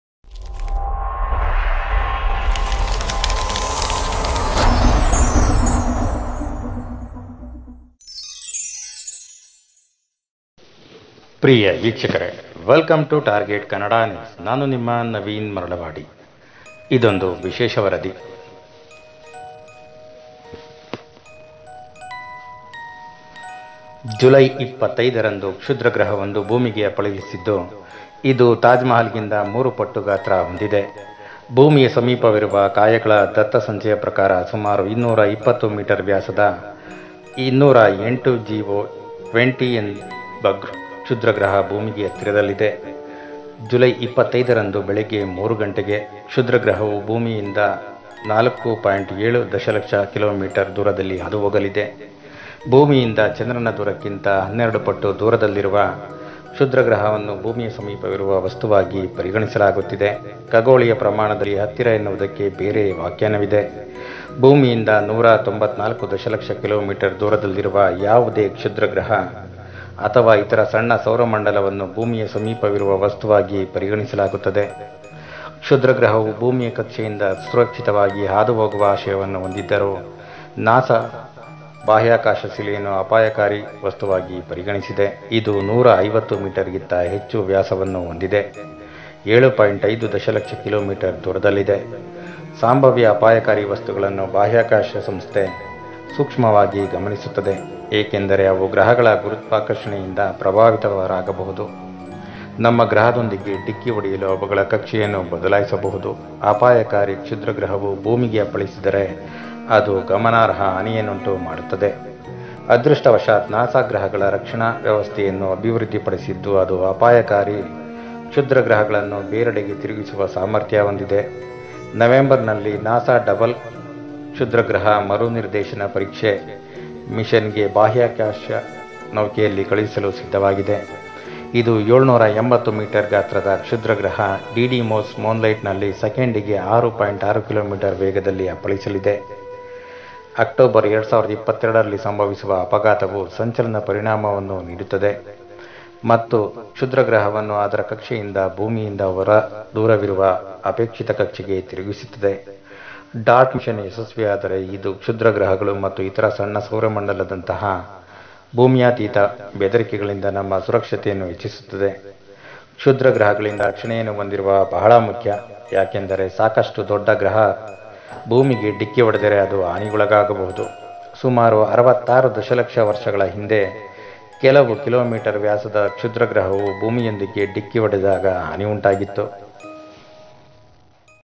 ಭೂಮಿಗೆ ಅಪ್ಪಳಿಸಲಿದೆ ಶಕ್ತಿಶಾಲಿ ಕ್ಷುದ್ರಗ್ರಹ..!Target Kannada News
ನಾಸಾ ಬಿಚ್ಚಿಟ್ಟ ಸತ್ಯ ಭೂಮಿಗೆ ಅಪ್ಪಳಿಸಲಿದೆ ಶಕ್ತಿಶಾಲಿ ಕ್ಷುದ್ರಗ್ರಹ ವಿನಾಶದ ಅಂಚಿನಲ್ಲಿ ಭೂಮಿ...